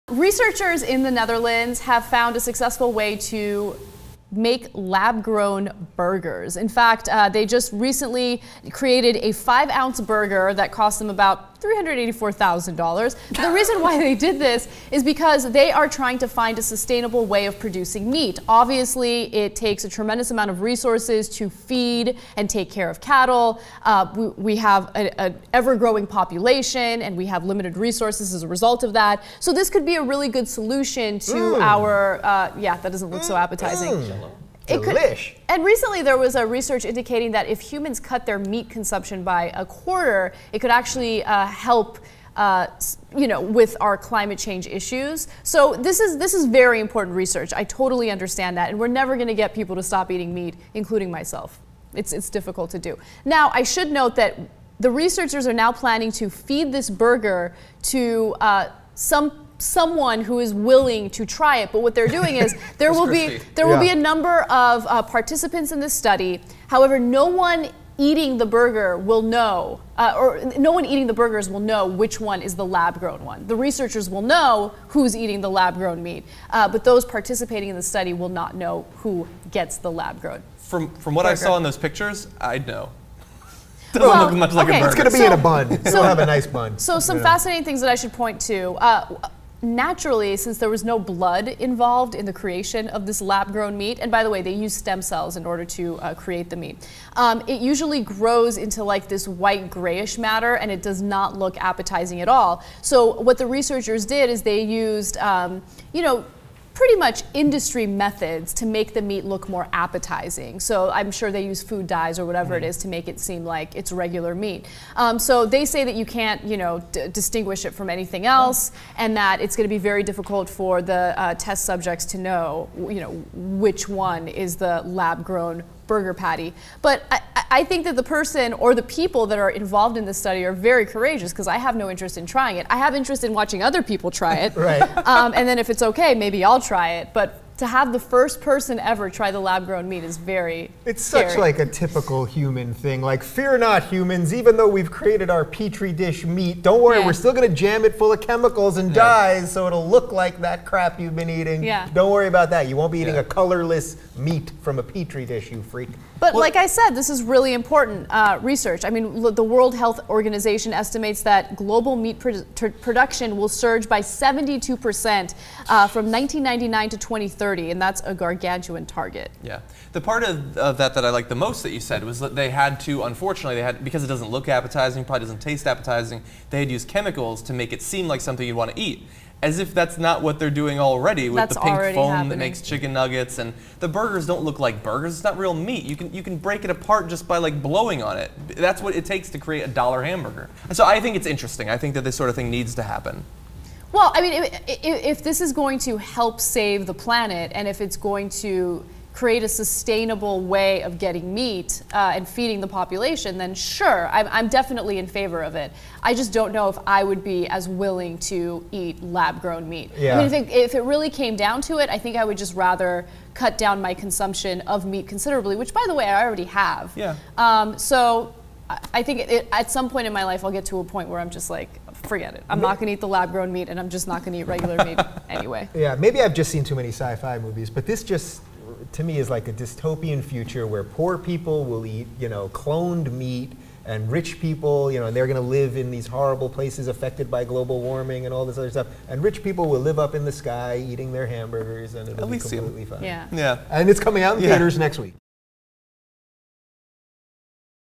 Would You Eat A Burger Grown In A Lab?
"The world's first test-tube hamburger, made from lab-grown meat, will be served in London next Monday, August 5.

The 5-ounce burger, created by Mark Post from Maastrict University in the Netherlands, will be fed to an unknown diner whose reaction will be seen in front an invitation-only crowd."*

Ana Kasparian, John Iadarola and Dave Rubin break it down.